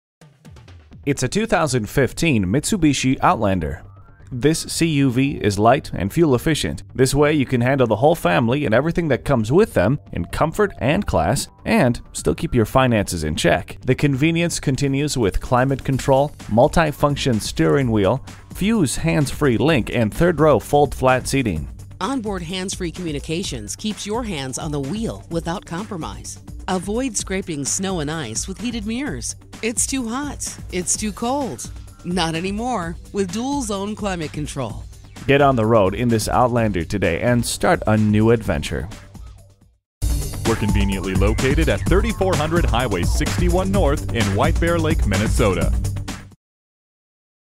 2015 Mitsubishi Outlander St-Paul White-Bear-Lake, MN #74149 - SOLD
Year: 2015
Make: Mitsubishi
Model: Outlander
Trim: 4WD 4dr SE
Engine: 2.4 L 4 Cyl. Cyl.
Transmission: Automatic
Color: Cosmic Blue
Stock #: 74149
VIN: JA4AZ3A39FZ010495

Address:
3400 Highway 61 N